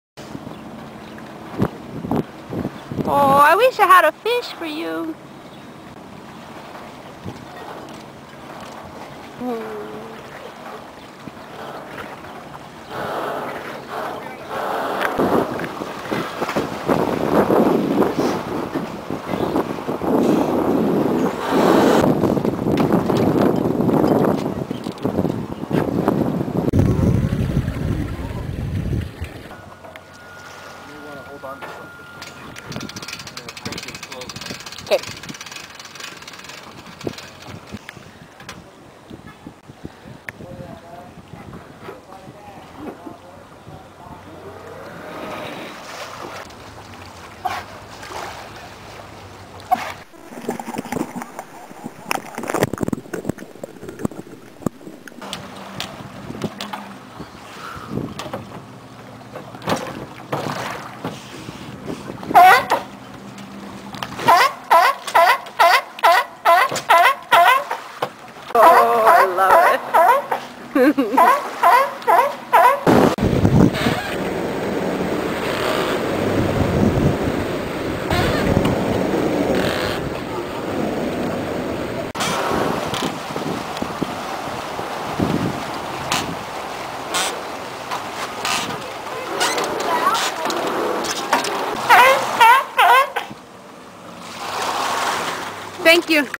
Seals on shelter Island....gimme some fish!!
this video is a little boring, but I love the seal sound a minute into...